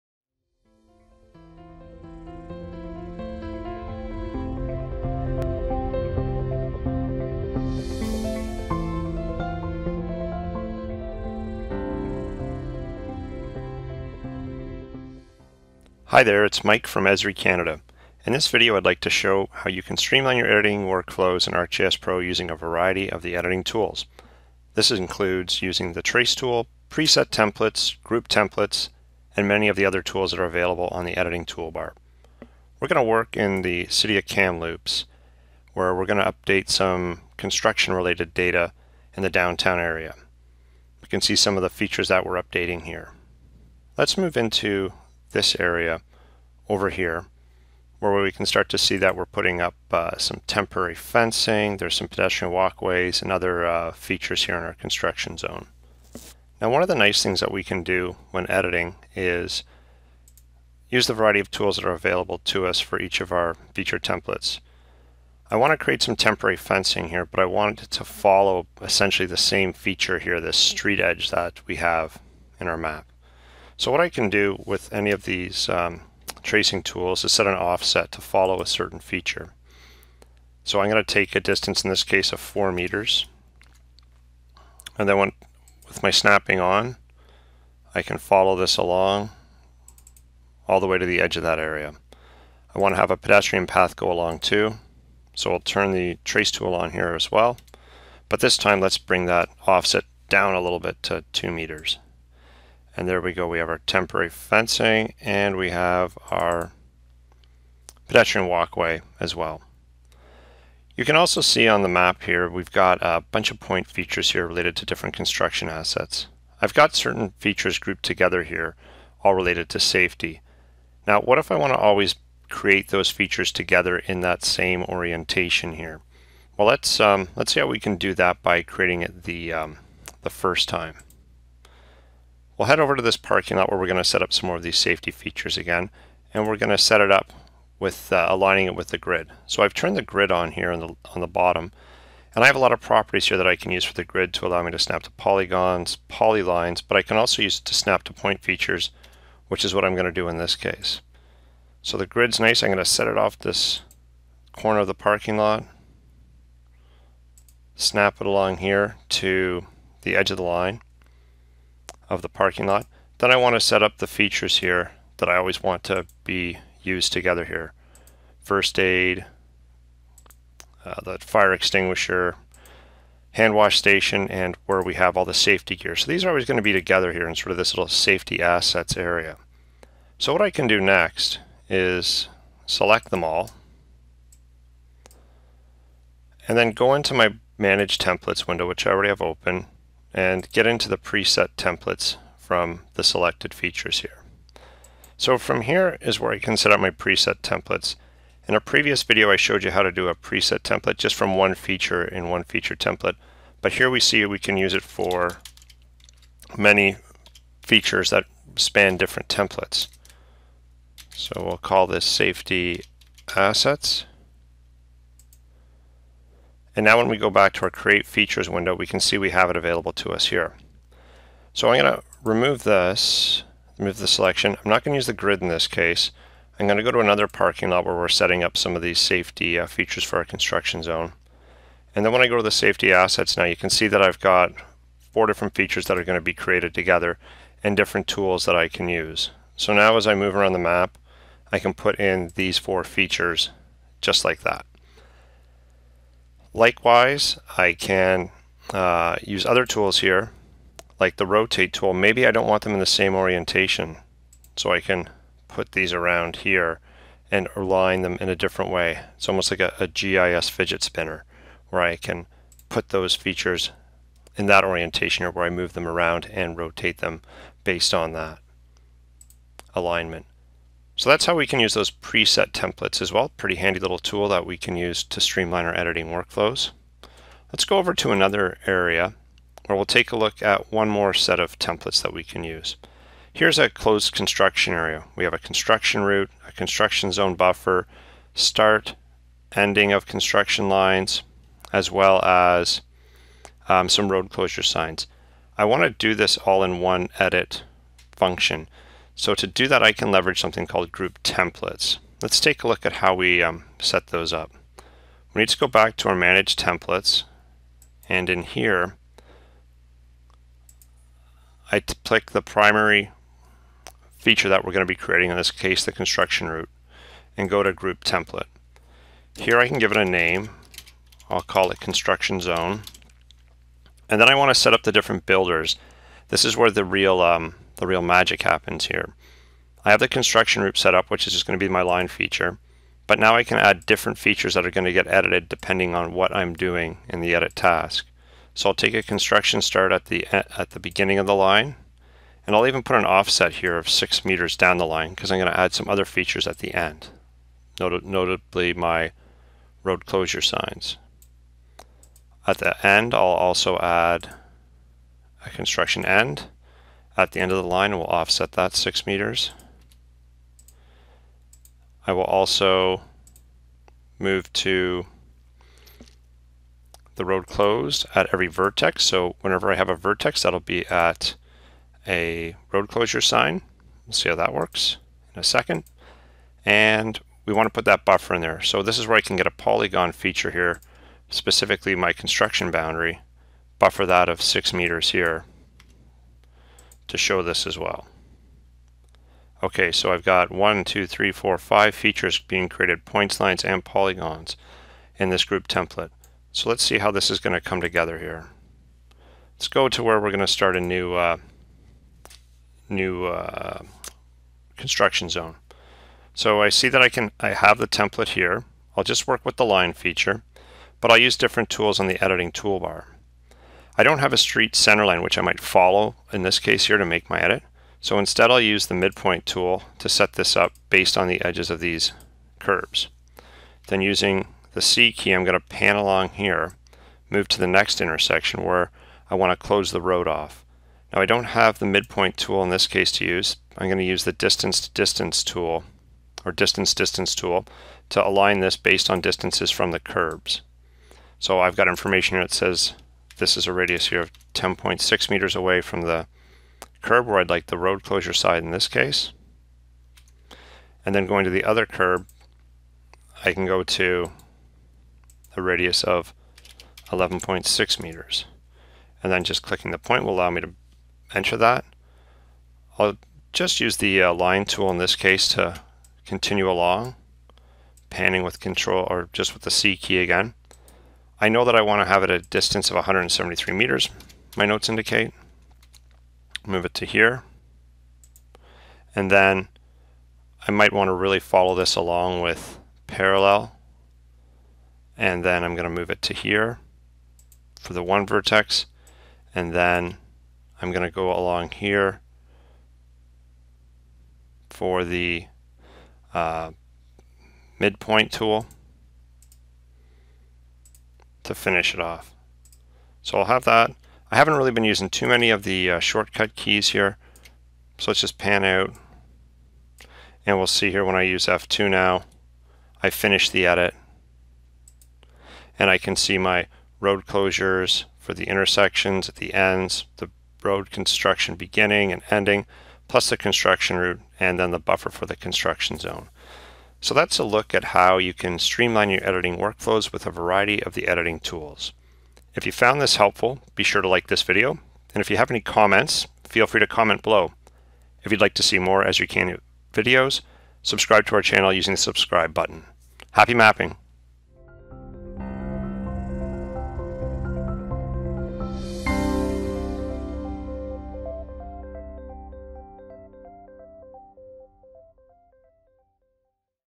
ArcGIS Pro Editing Tip: Streamline Your Editing Workflows
Do you edit data in ArcGIS Pro and find that you are performing tasks that seem a bit repetitive? Would you like to streamline your workflows to make editing your data more efficient? There are many tools and templates available to you in the editing environment that can help you reduce the number of clicks while editing and maintain data accuracy. In this video we show how you can use Trace, Preset Templates, Group Templates and many of the tools on the Editing Toolbar to streamline your editing workflows.